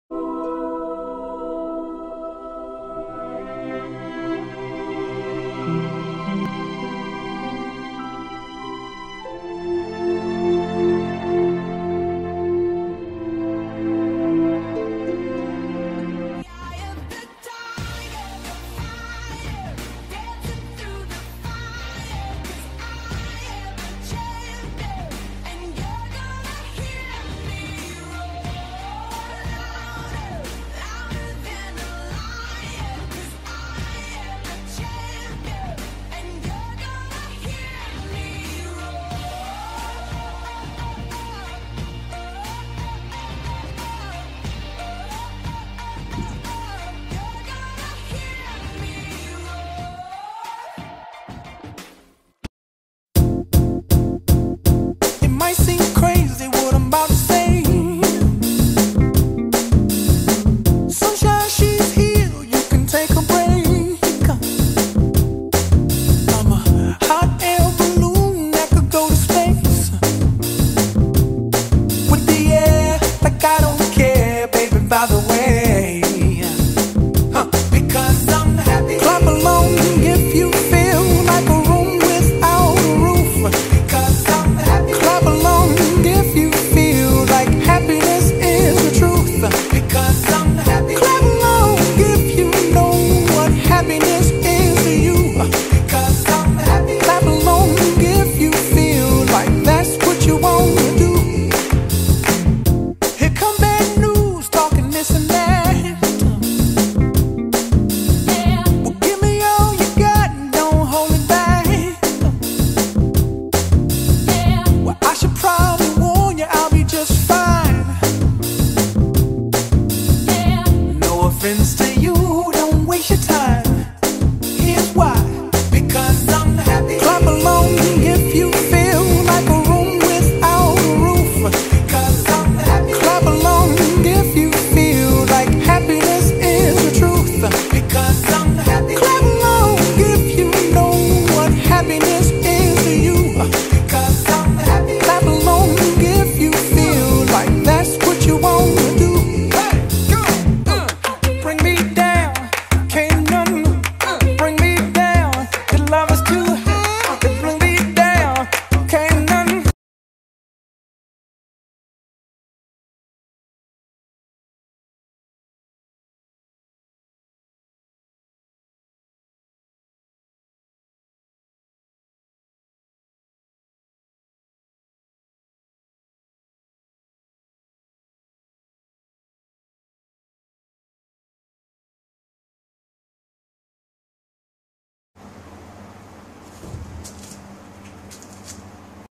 Bobby the kitten _ up for adoption
Bobby is a gorgeous Arabian Mau looking for his forever loving family. This is his story so far.
DOB: 22 Aug 2013